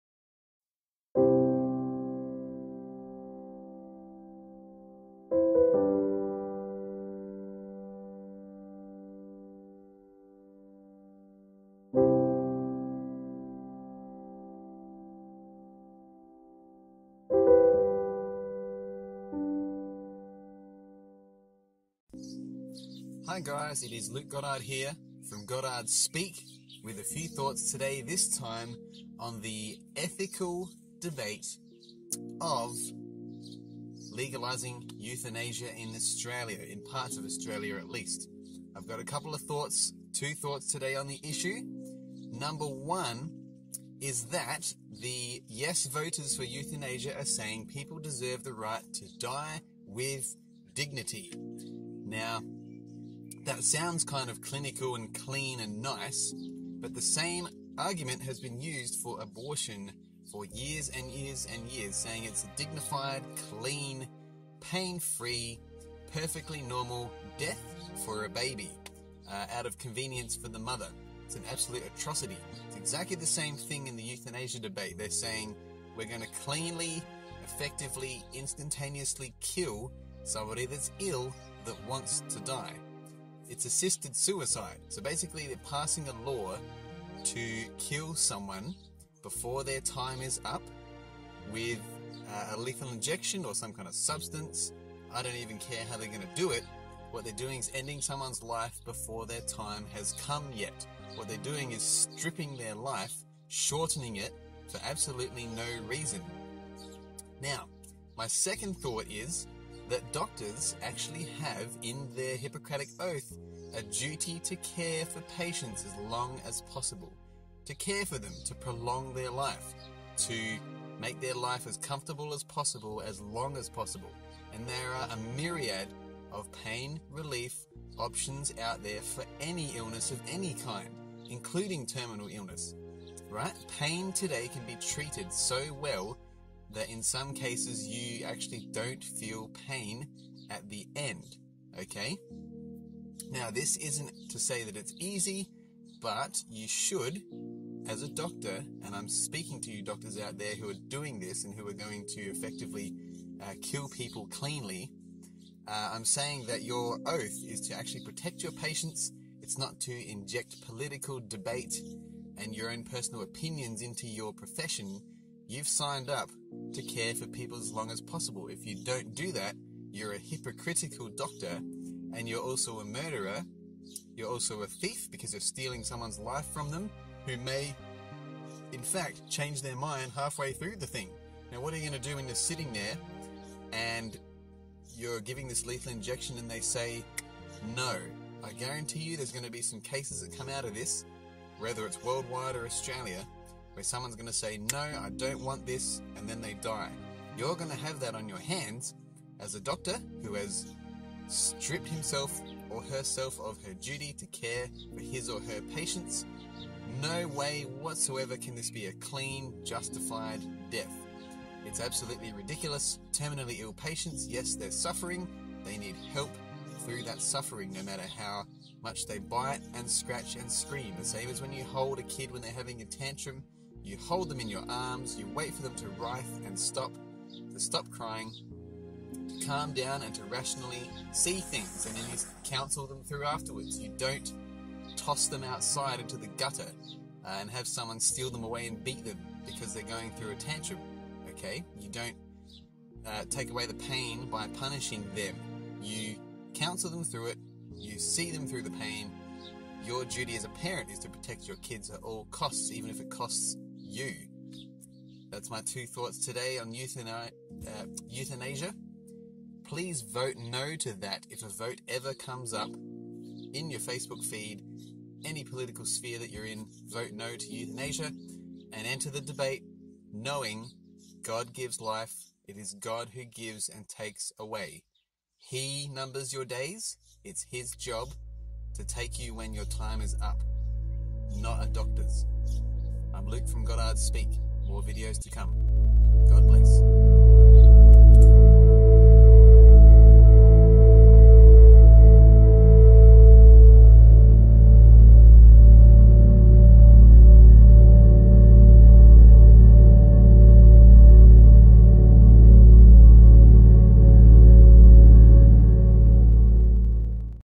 Australia is dangerously close to voting through a Euthanasia bill this year. You can stop it
Australia has debated euthanasia in parliament alot recently, putting it to a vote in Victoria this month. Australia has also been debating the issue hotly, as it cuts to the heart of what people so desperately want - to stay alive. Yet there are hundreds of people who literally want doctors to be able to have the power to inject terminally ill patients who so desire lethally, without ramifications or prosecution. This sickening truth is the exact same legal process that occurs when a baby is killed in a womb for comfort and convenience purposes for an expectant mother who doesn't want their child. This vote in Australia has pushed me to write and post this video on euthanasia, and to bring up an issue that, though delicate, must be addressed rationally and with care.

You can stop euthanasia. And the time to do it is ALWAYS!